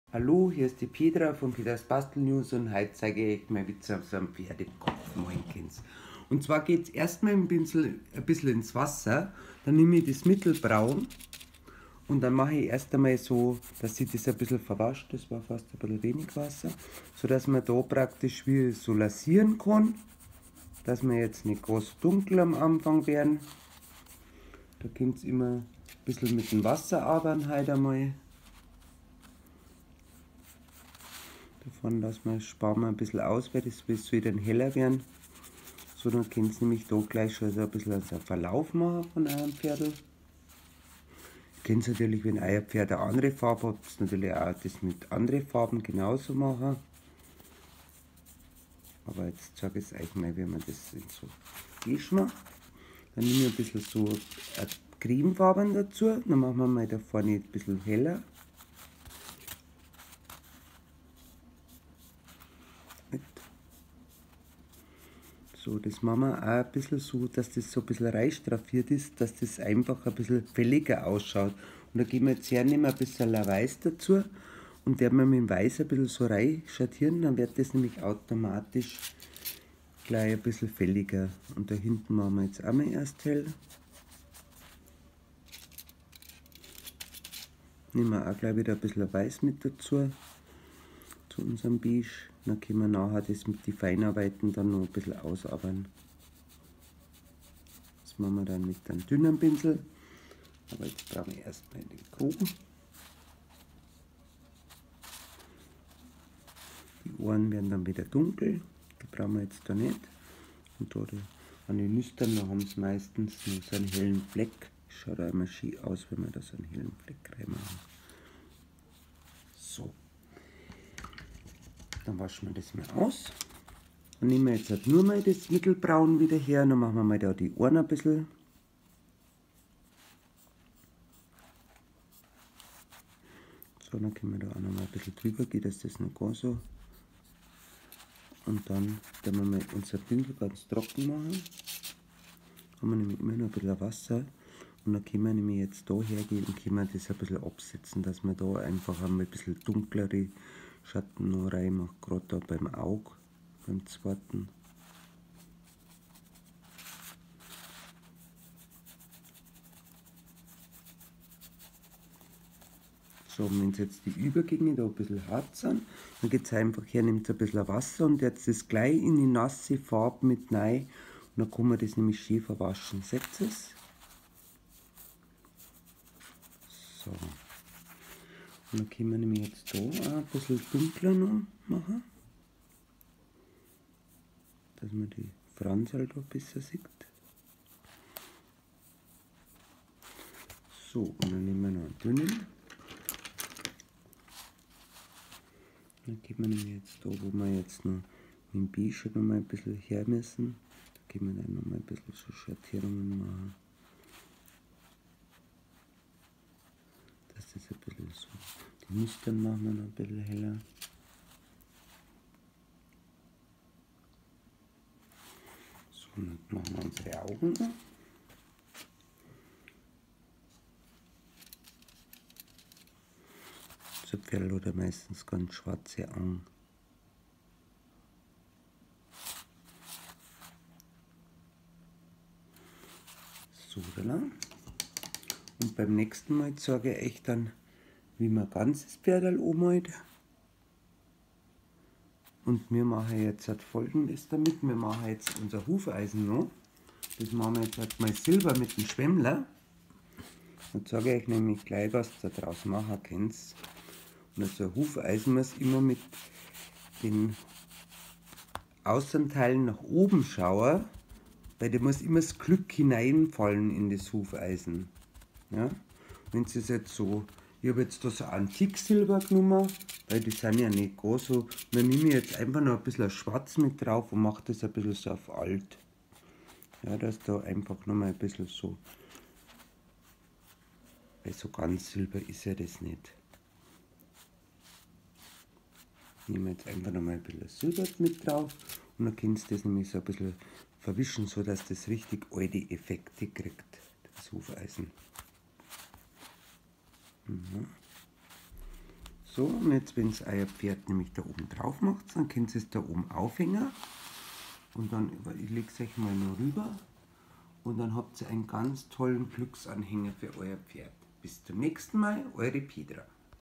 Pferdekopf aus Holz anmalen mit Acrylfarbe
Petra malt für euch heute einen Pferdekopf an. Sie zeigt euch wie ihr tolle Verläufe mit den Acrylfarben bekommt.
Die Pferdeköpfe, Pferde, Hufeisen, Steigbügel und vieles mehr gibt es bei uns im Shop: Pferde;